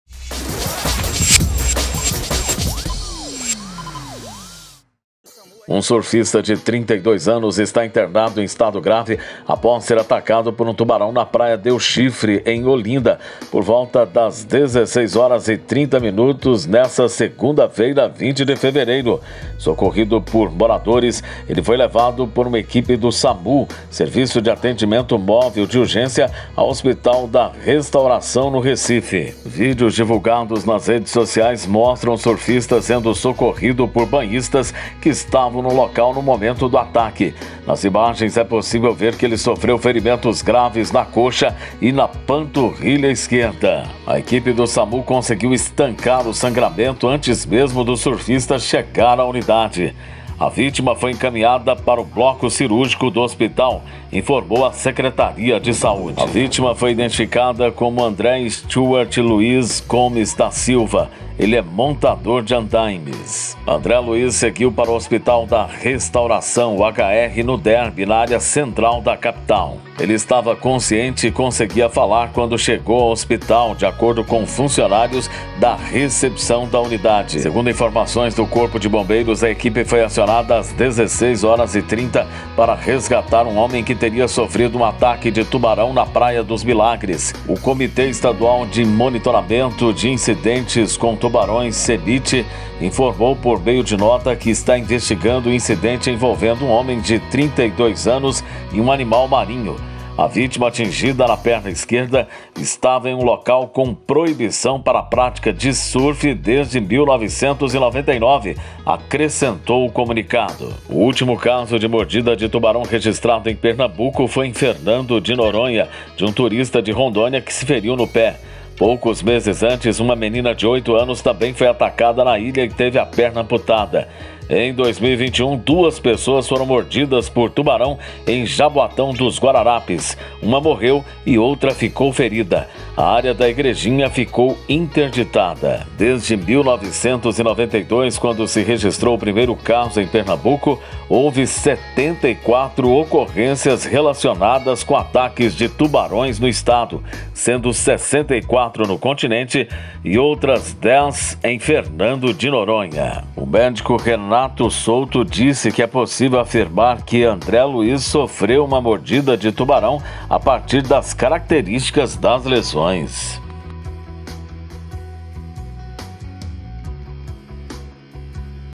SURFISTA SOFRE ATAQUE DE TUBARÃO EM OLINDA, EM PERNAMBUCO!
Um surfista foi atacado por um tubarão, nesta segunda (20), em Olinda. Socorrido por populares, ele apresentava ferimentos graves na perna.

O médico Renato Souto disse que é possível afirmar o homem, de 32 anos, sofreu uma mordida de tubarão a partir das características das lesões.

"Foi uma mordedura de tubarão bem profunda. Houve perda de muito sangue", atestou o médico, em entrevista à TV Globo.

A vítima foi identificada como André Luiz Gomes da Silva. Montador de andaimes, ele teve ferimentos na coxa e na panturrilha esquerdas.

André Luiz seguiu para o Hospital da Restauração (HR), no Derby, na área central da capital. Segundo a emergência, ele deu entrada por volta das 17h10 e foi levado para o bloco cirúrgico, em estado grave.

O homem estava consciente e conseguia falar quando chegou ao hospital, de acordo com funcionários da recepção da unidade.

Até a última atualização desta reportagem, às 22h, André Luiz ainda estava na cirurgia, segundo a assessoria de comunicação do HR.

Um surfista de 32 anos está internado em estado grave após ser atacado por um tubarão na praia Del Chifre, em Olinda, por volta das 16h30 de hoje. Socorrido por populares, ele foi levado por uma equipe do Samu ao Hospital da Restauração, no Recife.

Nas imagens, é possível ver que ele sofreu ferimentos graves na coxa e na panturrilha esquerda.

"A equipe do SAMU conseguiu estancar o sangramento antes mesmo do rapaz chegar à unidade. A vítima está, neste momento, no bloco cirúrgico do hospital", informou a secretaria de saúde.

Histórico de ataques de tubarão em Pernambuco. Desde 1992, quando começaram a ser contabilizados, foram notificados outros 67 incidentes com tubarão em Pernambuco. Os dados são do CEMIT (Comitê Estadual de Monitoramento de Incidentes com Tubarões. Ao todo, houve 26 mortes, neste período.

Um surfista de 32 anos foi mordido por um tubarão na tarde desta segunda-feira (20), na praia Del Chifre, em Olinda, na Região Metropolitana do Recife.

Uma viatura de resgate e um bote inflável de salvamento com guarda-vidas do Corpo de Bombeiros foram enviados ao local por volta das 16h30, mas o homem já havia sido retirado do mar por populares.

O Serviço de Atendimento Móvel de Urgência (Samu), que também foi acionado e prestou os primeiros atendimentos, informou que o caso foi um "provável incidente com tubarão". A vítima foi encaminhada para o Hospital da Restauração (HR), na área central do Recife.
Segundo o HR, o surfista, que não teve o nome divulgado, deu entrada na unidade hospitalar em estado grave e se encontra no bloco cirúrgico.
Por meio de nota, o Comitê Estadual de Monitoramento de Incidentes com Tubarões (Cemit) informou que o caso ocorreu na praia de Milagres, envolveu um animal marinho e está sendo analisado. O Cemit também disse que o local possui proibição para a prática de surf desde o ano de 1999.
Desde 1992, quando foi registrado o primeiro caso em Pernambuco, foram notificadas 74 ocorrências envolvento ataques de tubarão, sendo 64 no continente e outras 10 na Ilha de Fernando de Noronha, de acordo com o Cemit.

O surfista André Luiz Gomes da Silva, 32 anos, atacado por um tubarão na Praia de Del Chifre, em Olinda, Região Metropolitana do Recife, na tarde desta segunda-feira (20) se encontra em estado grave. Ferido na panturrilha da perna esquerda, ele passou por cirurgia no Hospital da Restauração.

Em entrevista à TV Globo, o médico Renato Souto, do Serviço de Atendimento Móvel de Urgência (Samu), afirmou que o caso de trata de uma mordedura de tubarão. “Bem profunda. Pegou musculatura e vasos também”. André Luiz surfava sozinho na praia. O Samu foi acionado minutos após a acidente, ocorrido por volta da16h30.

Ainda na praia, a vítima, retirada da água por banhistas e pessoas que trabalhavam no local, recebeu os primeiro socorros e foi encaminhado ao HR, onde deu entrada por volta das 17h30 desta segunda-feira e passou por cirurgia.

Em caso de confirmação oficial da mordedura, este seria o caso 75 de ataques de tubarão em praias de Pernambuco desde 1992, ano em que o Comitê Estadual de Monitoramento de Incidentes com Tubarões (Cemit) fez o primeiro registro.

O Cemit, em nota, afirmou que está analisando o caso de Del Chifre. O comitê informa que 64 das 74 ocorrências registradas no estados aconteceram no continente e as demais no arquipélago de Fernando de Noronha.